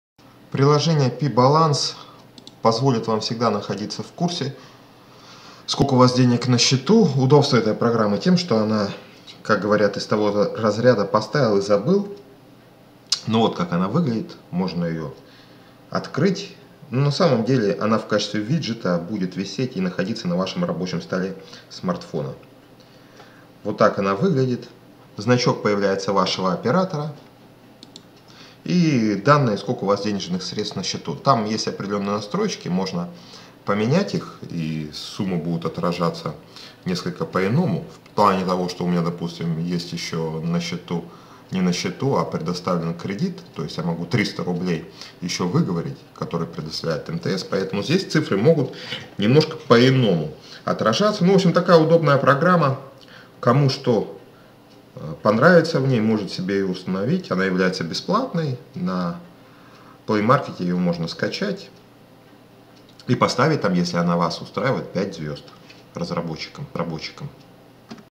piBalance контроль баланса,Android
Описание
Будьте всегда в курсе вашего баланса. Приложение запрашивает баланс автоматически и показывает его в виде виджета на рабочем столе. Работает по принципу "поставил и оставил".

Вместо баланса можно следить за количеством бонусов, суммой долга, лимитом интернета, sms и пр.

Обновляется автоматически:
- после завершения разговора
- после отключения от интернета
- периодически с заданной частотой
Приложение работает посредством отправки бесплатных смс запросов а не через USSD команды вида *100#. Но не все операторы поддерживают такую услугу, поэтому Билайн и Теле2 в России не поддерживаются. В Украине не поддерживаются МТС и абоненнты предоплаченной связи Киевстара